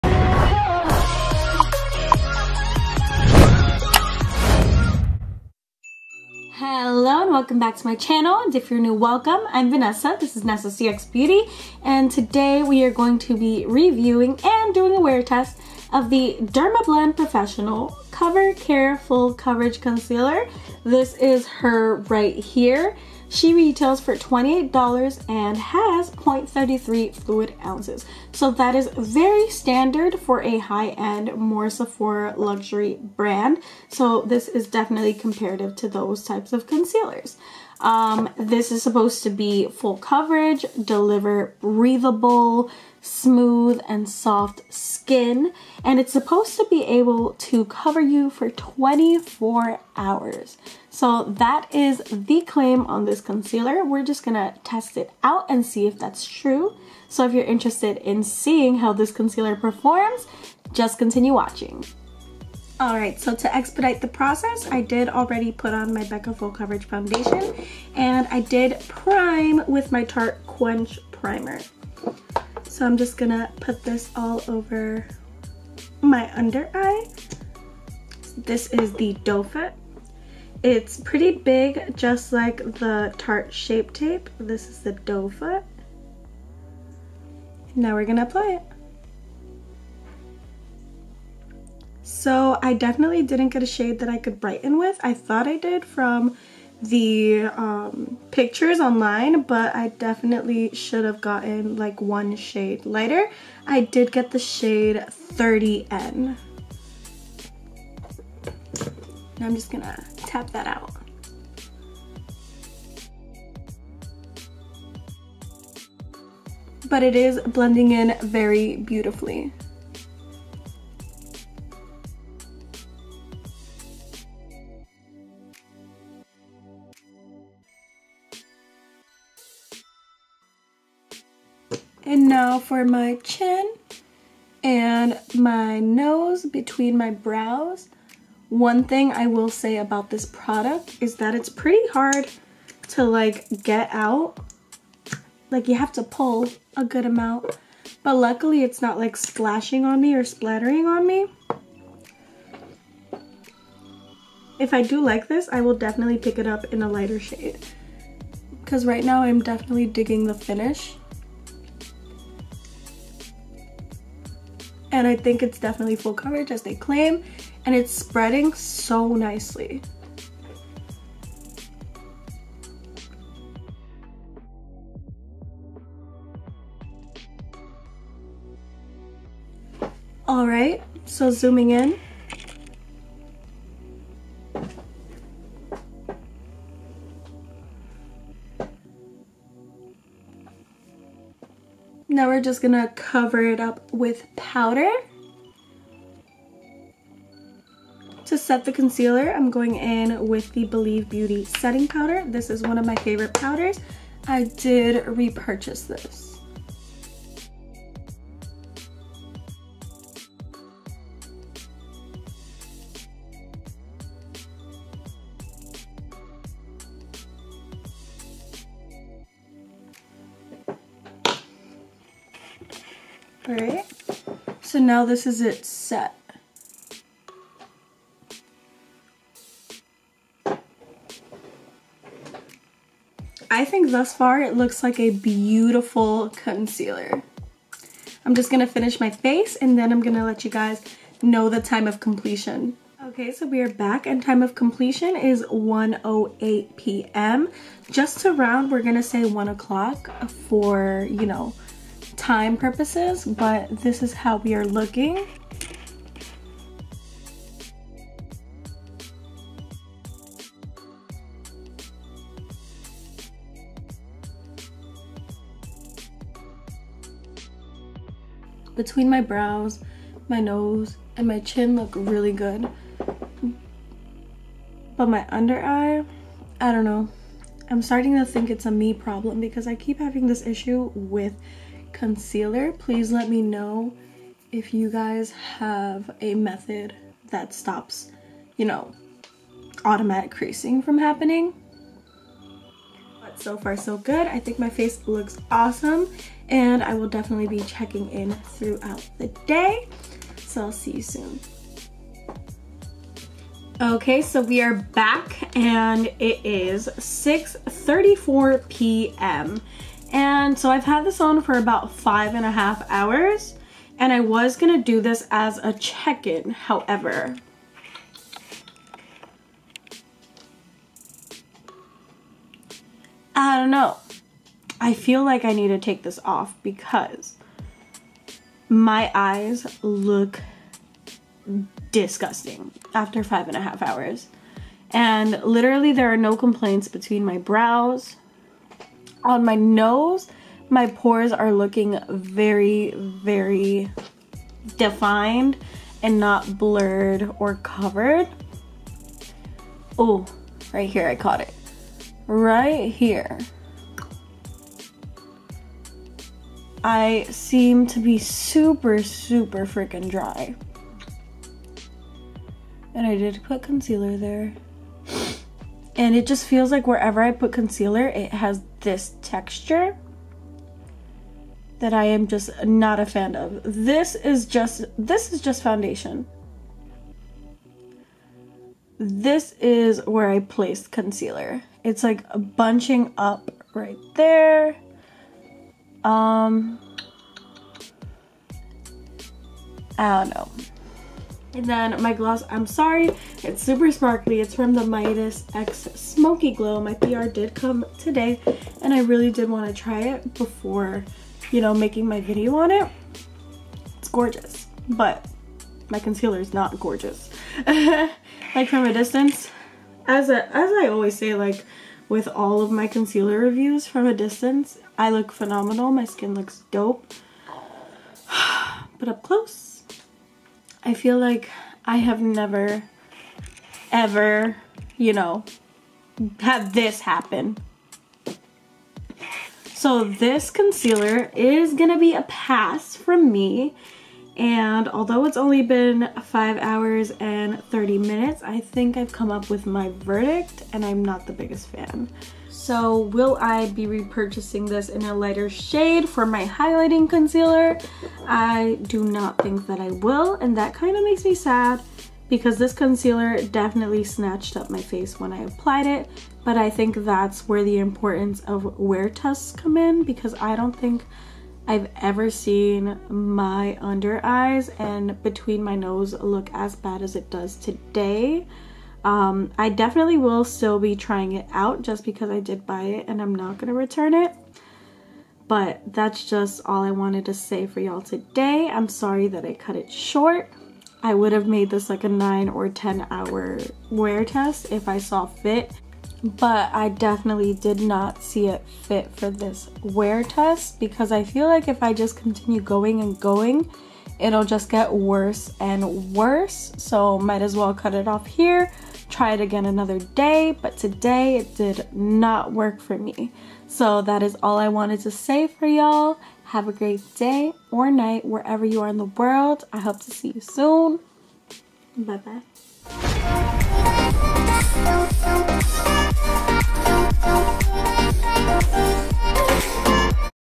DERMABLEND COVER CARE FULL COVERAGE CONCEALER | WEAR TEST & REVIEW
Introducing Dermablend’s brand new concealer! The dermablend cover care full coverage concealer delivers full coverage, hydrating, 24 hour wear. Let’s see if the cover care full coverage concealer lives up to its claims.

& use code: NESSACXBEAUTY to receive $5.00 to shop on poshmark